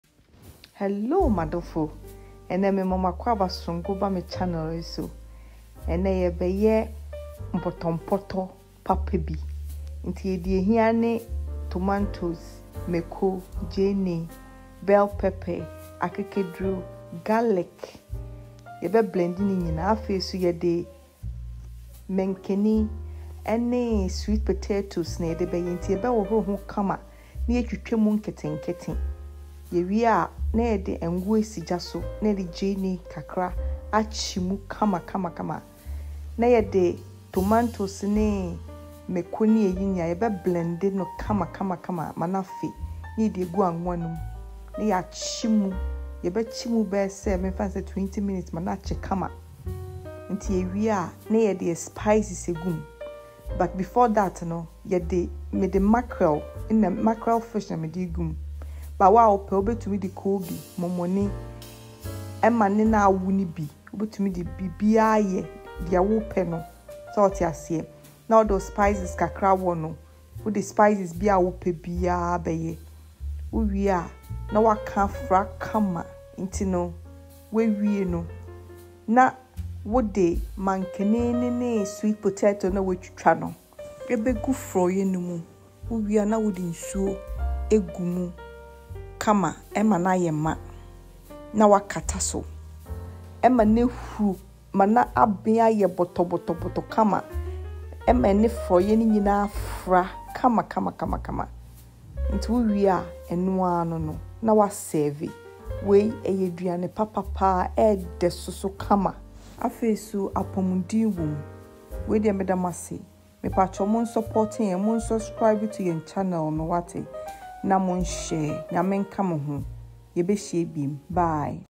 MPOTOMPOTO GHANAIAN RECIPE || COCOYAM AND SWEET POTATO PORRIDGE
This is Ghanaian local delicacy, tasty, healthy and delicious recipe

ingredients

Sweet potato
Coco yam
Fish
Palm oil
Spices
Vegetables